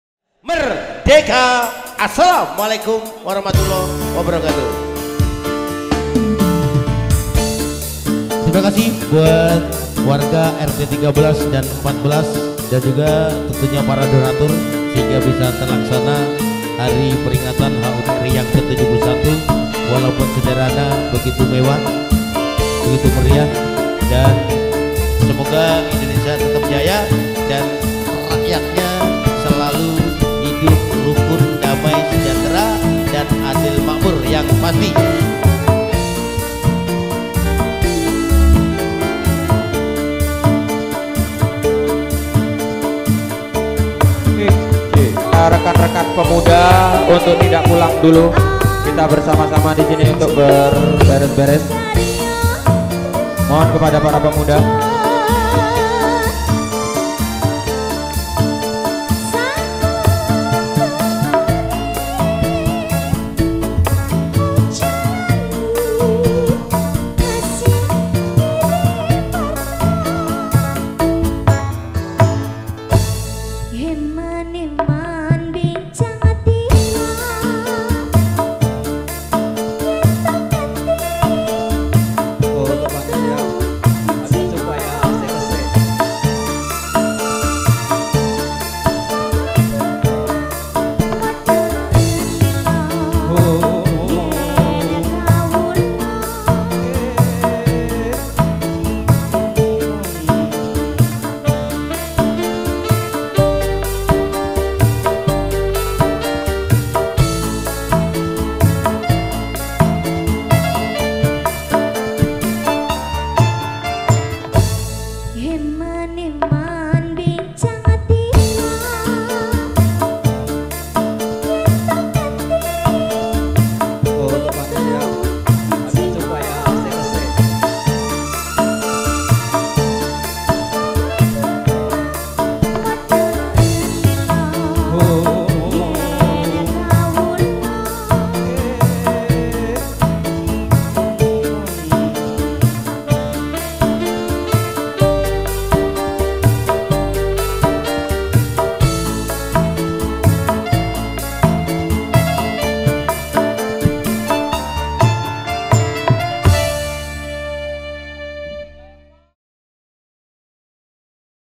Susunan Panitia HUT RI di Bukit Biru, 17 Agustus 2016
Semangat HUT RI di kel. Bukit Biru, Tenggarong, Kutai Kartanegara, Kalimantan Timur. dalam setiap pelaksanaanya dilakukan oleh kedua RT, Yakni RT 13 dan RT 14 Kelurahan Bukit biru yang tergabung dalam Ikatan Pemuda Marga Utama Bhakti ( IPMUB ) Surakarta.